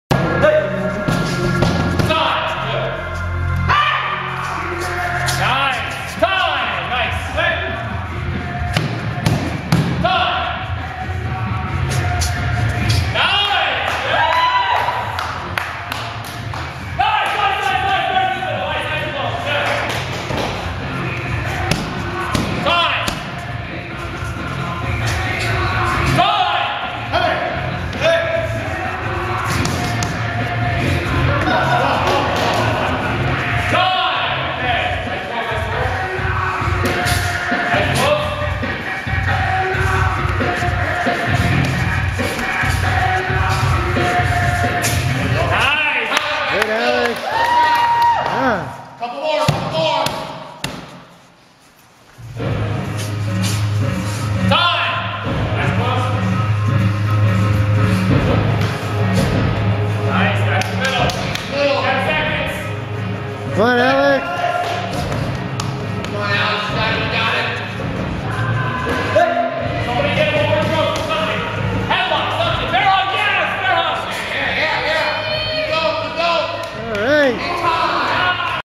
Circle of Death drills at Krav Maga Reborn
Krav Maga Reborn level 2 circle of death drill where you stand in the circle of attackers with your eyes closed for about 1.5 minutes. The drill is to practice recognition of an attack and to push stress cardio to the limits.

Krav Maga Reborn instructors are doing an awesome job pushing us to our limits while have too much fun!